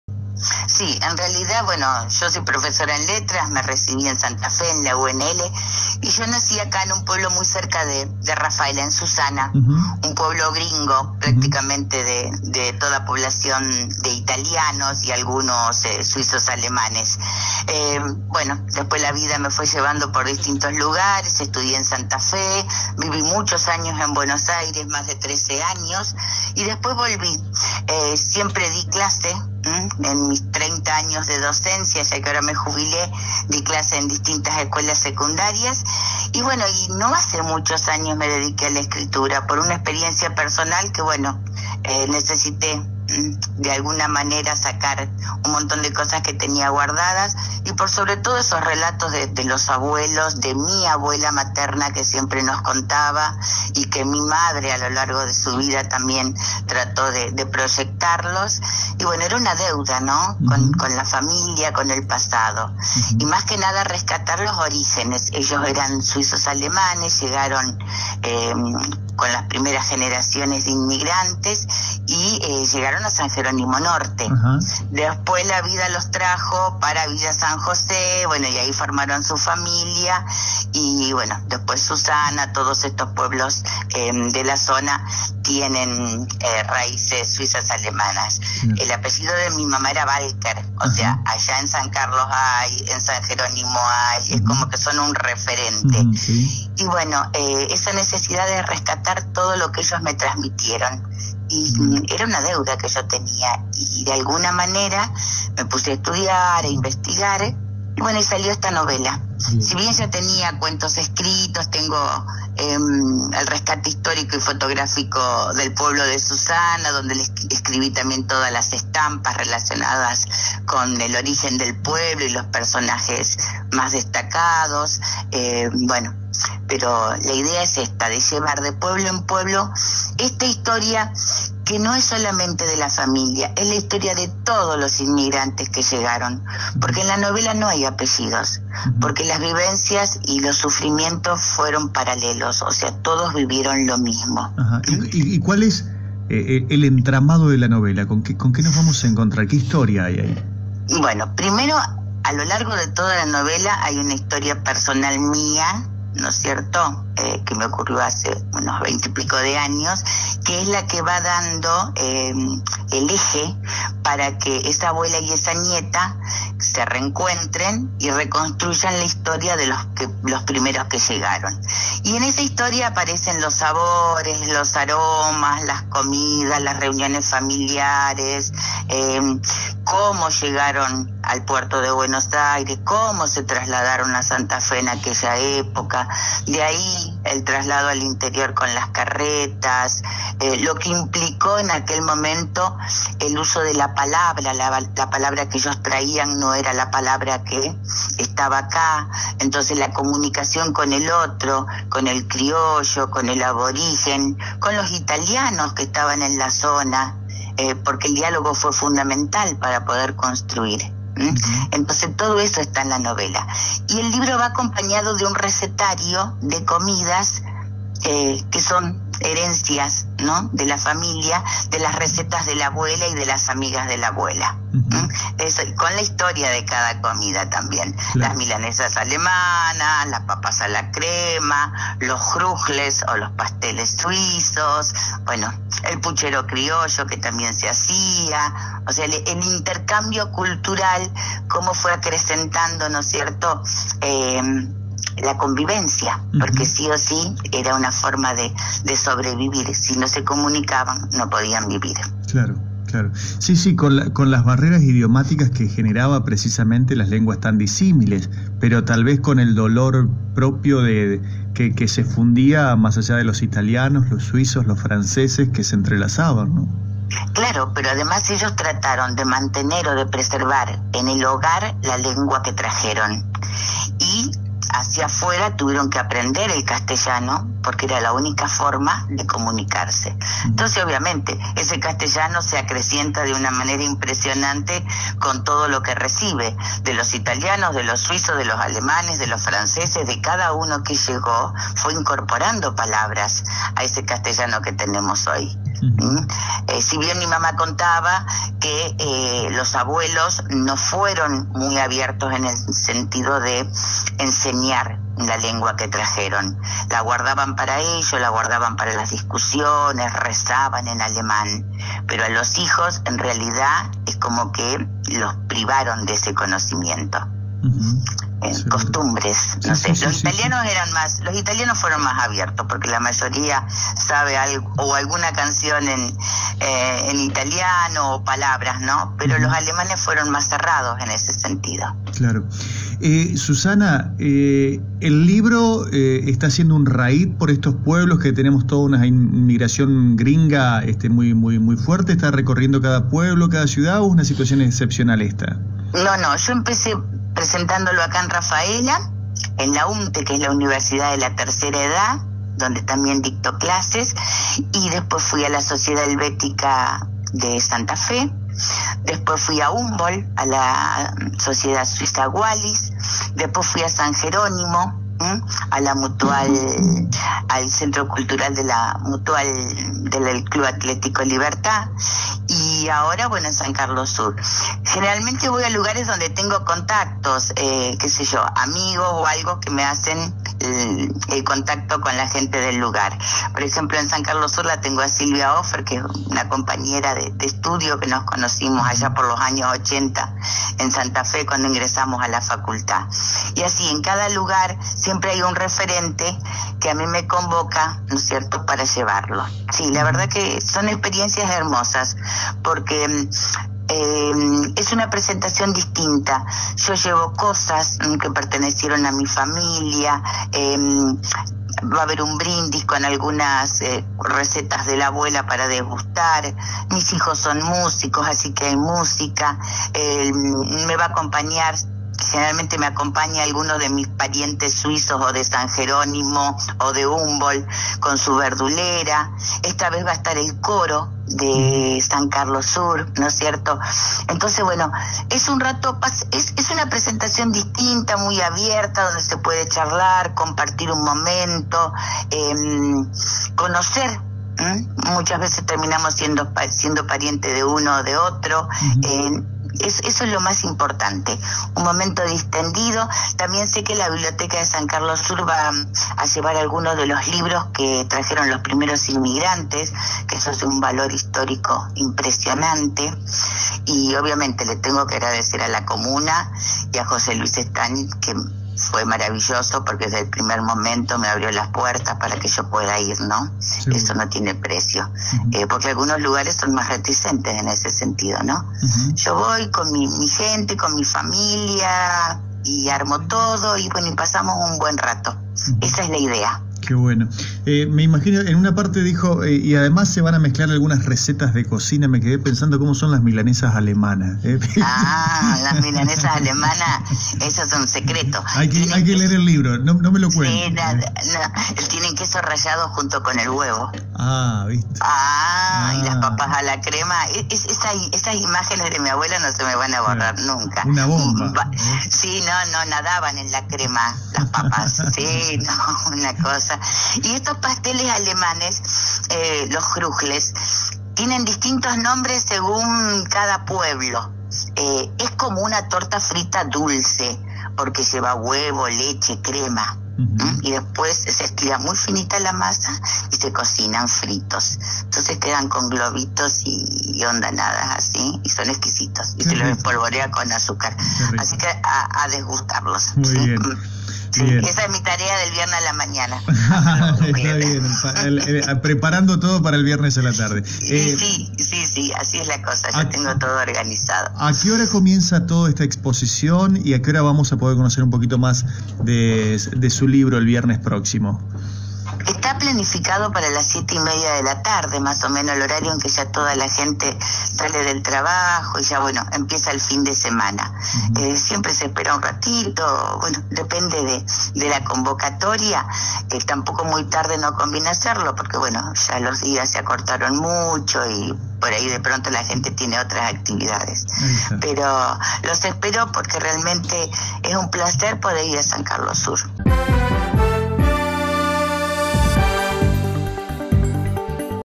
SUSANA MERKE – ESCRITORA
EL VIERNES PRESENTARÁ SU LIBRO “LAS VOCES DEL PASADO NO MUEREN”, EN S. C. SUD